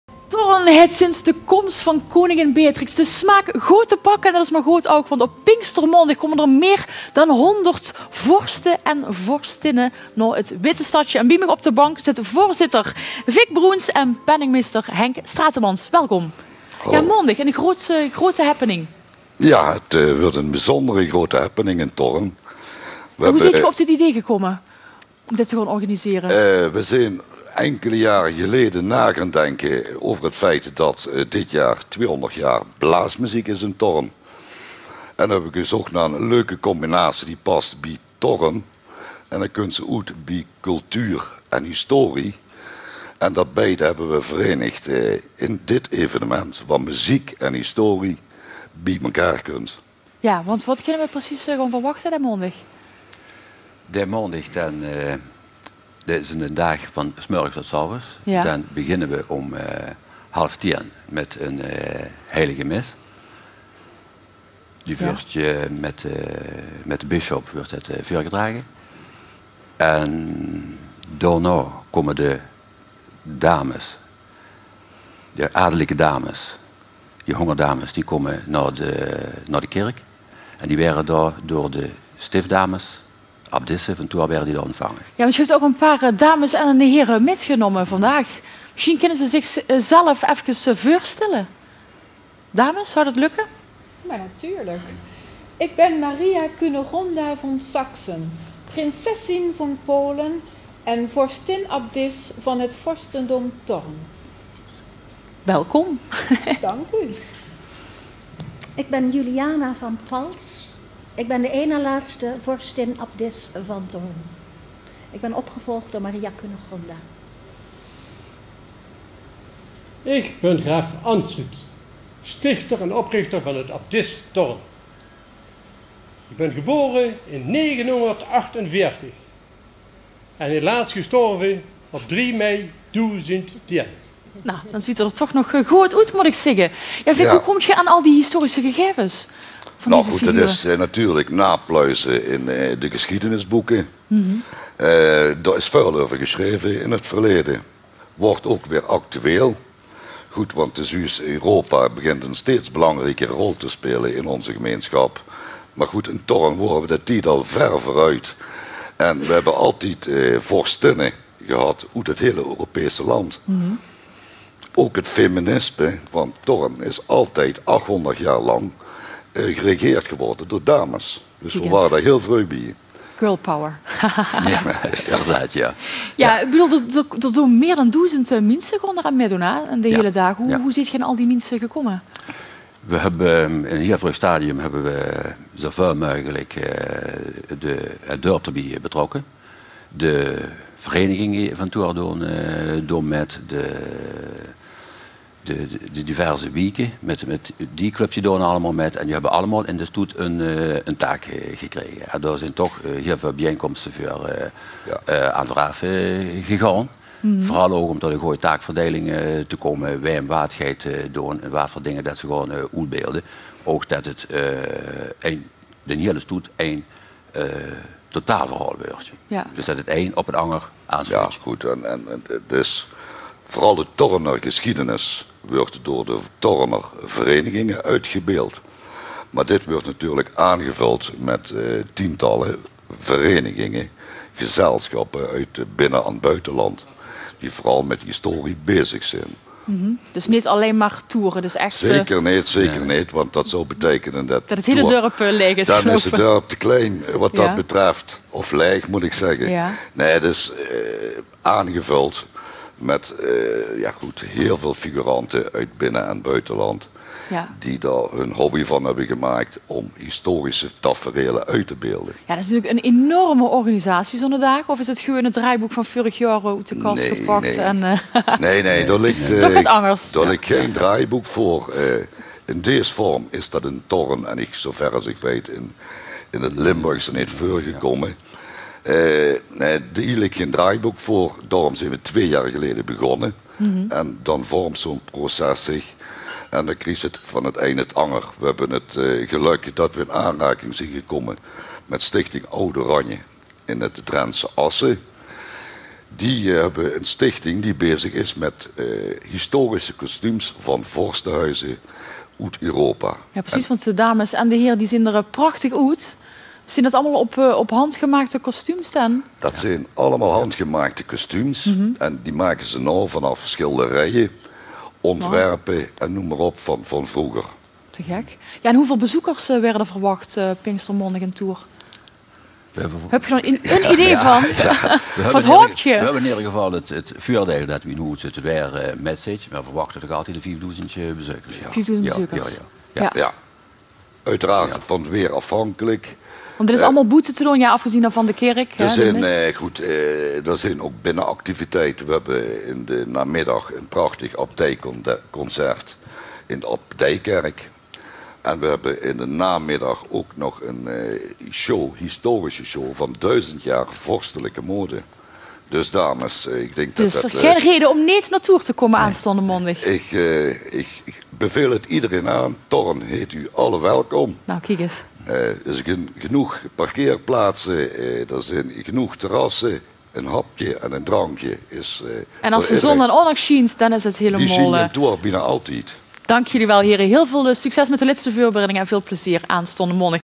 Grootste historische staatsbezoek aller tijden
Pinkstermaandag zullen de circa 30 vorstinnen die Thorn door de eeuwen heen hebben geregeerd het 'Grootste Historische Staatsbezoek' aller tijden ontvangen in hun Abdijkerk. Meer dan 100 gekroonde Staatshoofden van Europese vorstenhuizen en hun hofhouding, gekleed in prachtige historische kostuums zullen een bezoek brengen aan het witte stadje Thorn. Voorzitter Vic Brouns en penningmeester Henk Stratermans (van de organiserende Stichting '200 jaar blaasmuziek in Thorn') weten er alles van doen hier kond van bij TV Limburg.